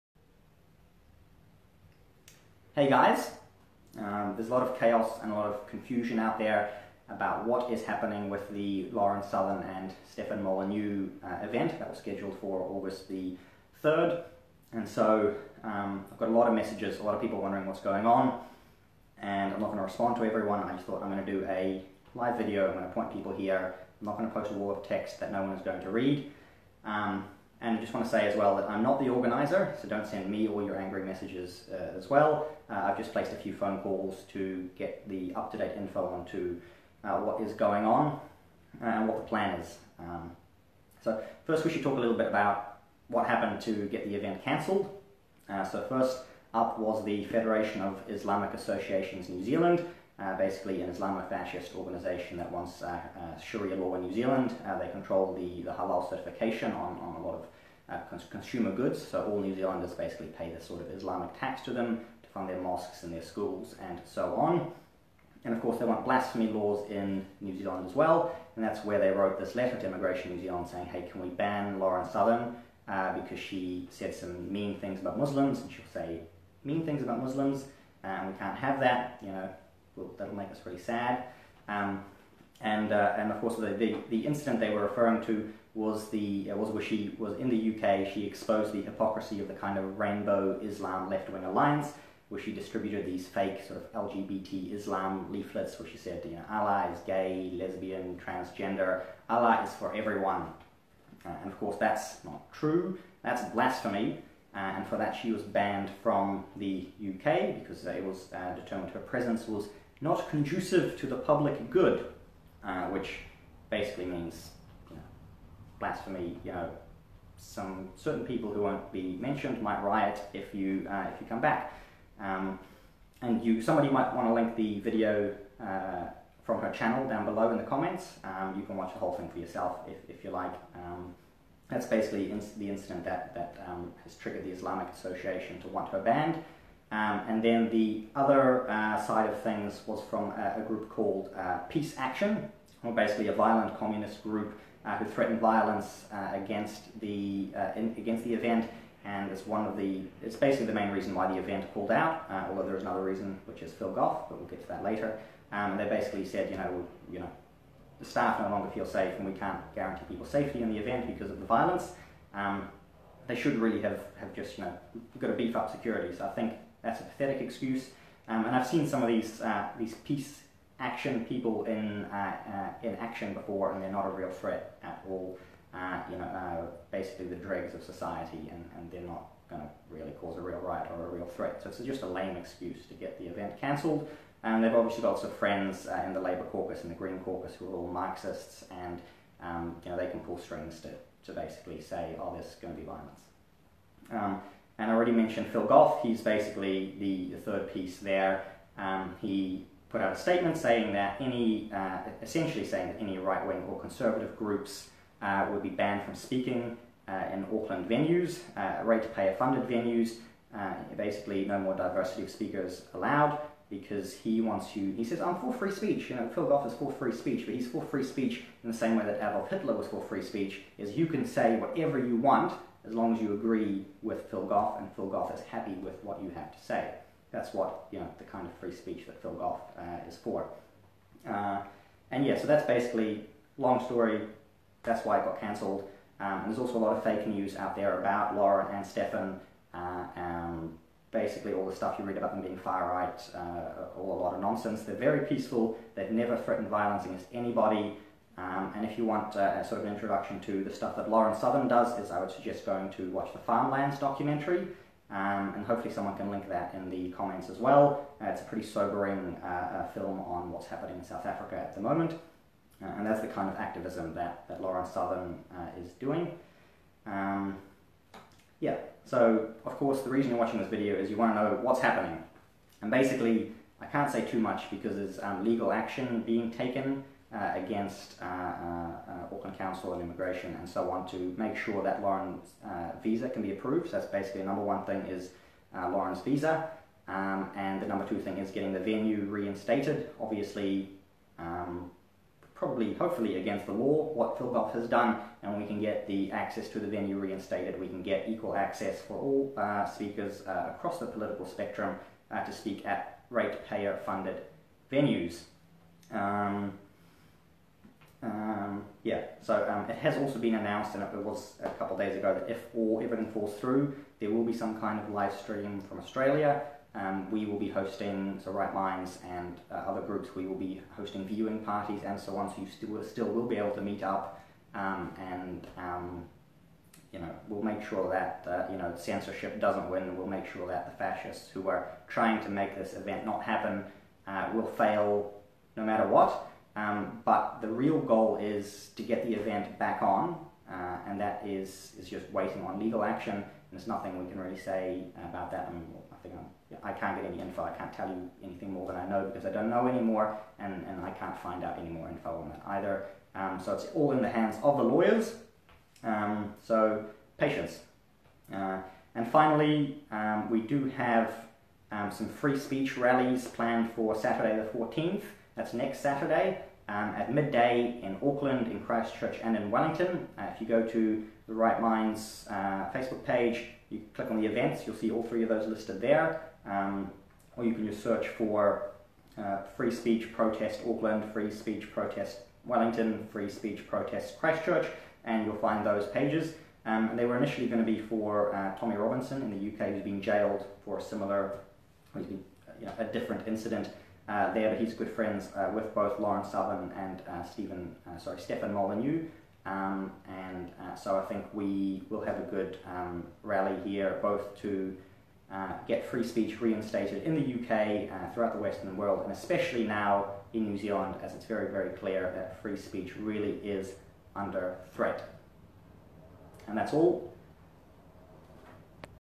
Update on the Lauren Southern situation
Lauren Southern was banned from New Zealand!

Uploaded from Facebook livestream on Saturday 7th July.